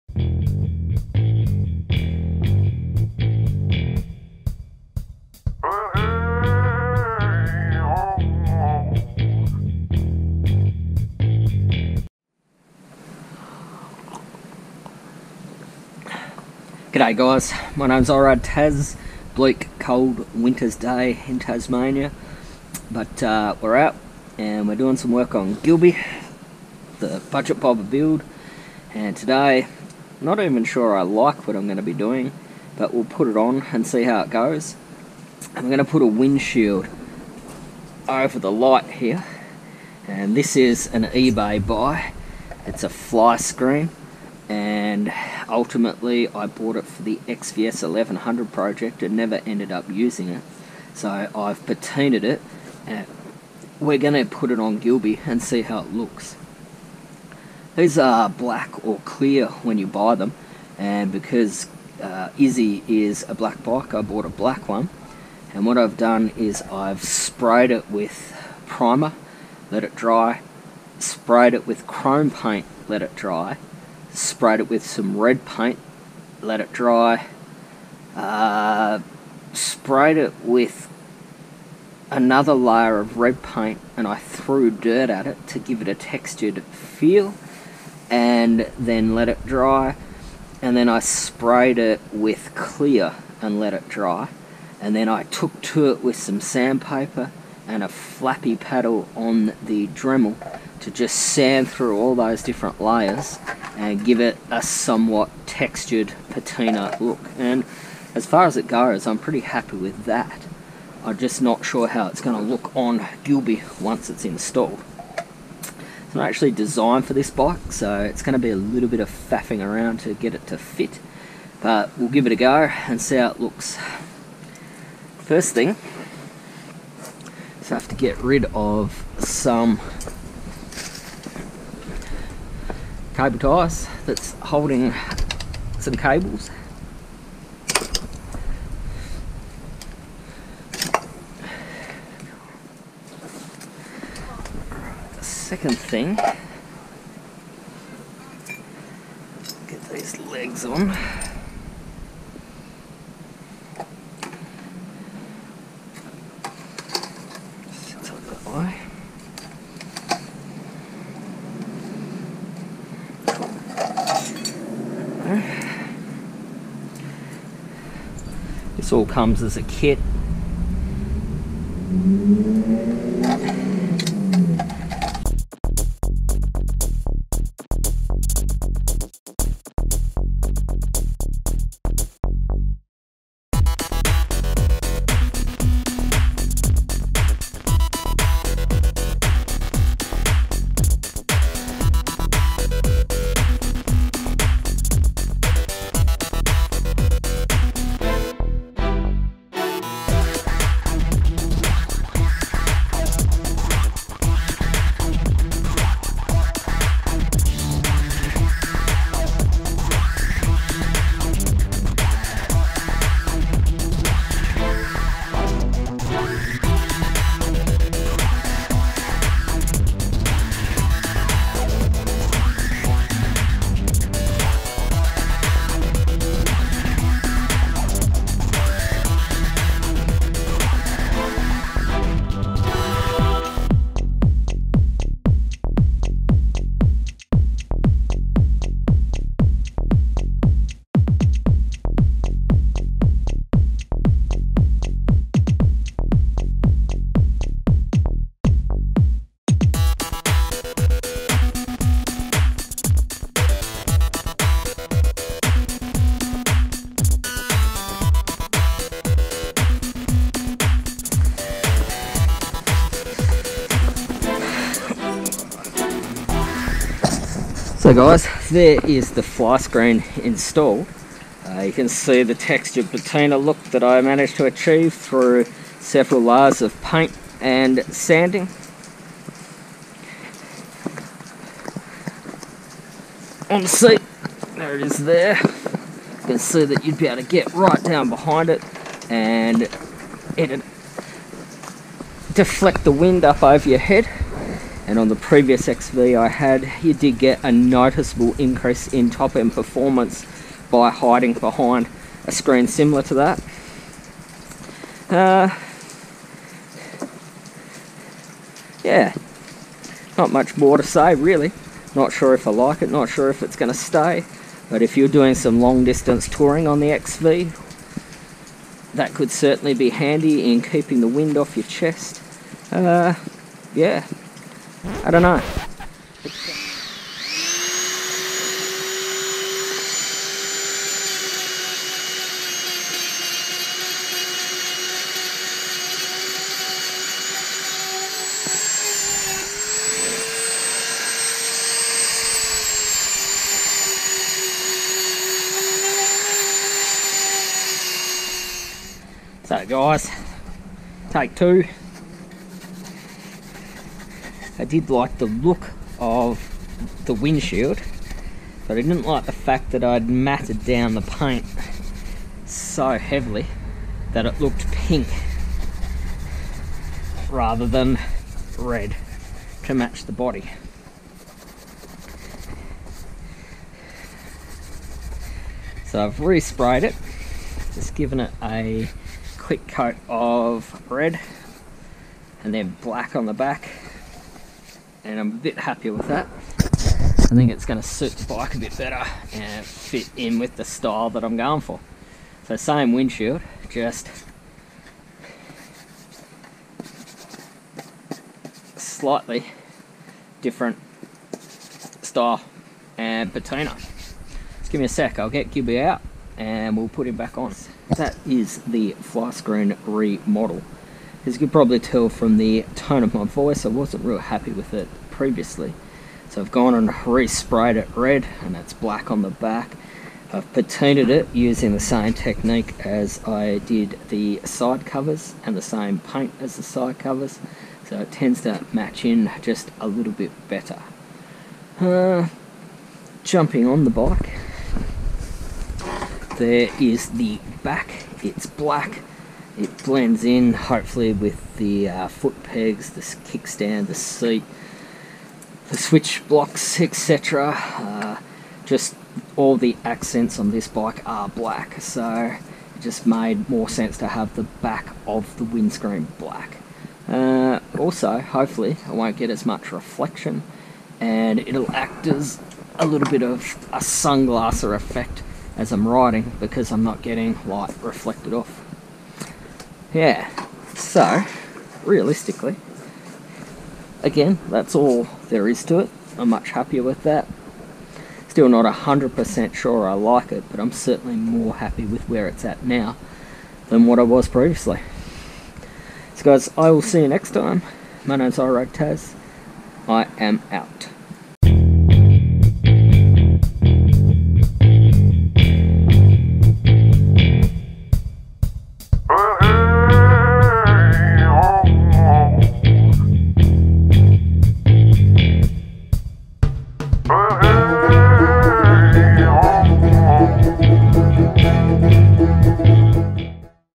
Budget Bobber Build #33 | Ebay or AliExpress Patina Fly Screen Wind Shield or Windscreen Install
In this episode we install an eBay or aliexpress flyscreen windshield or wind screen on Gilby the Yamaha XV250, Virago 250, V-star 250 or Route 66.  I have applied patina or fauxtina to the plastic by painting several layers of paint then sanding randomly through them.